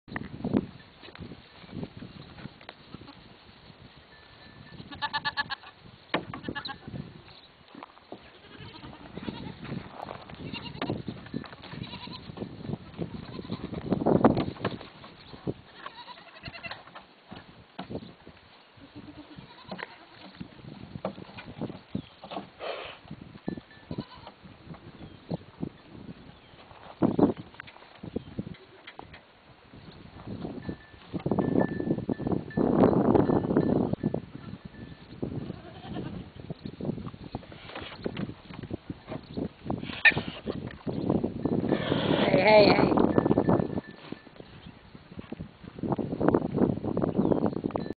Goats playing
Goats love to play , got some of all the other critters that share the pen with the goats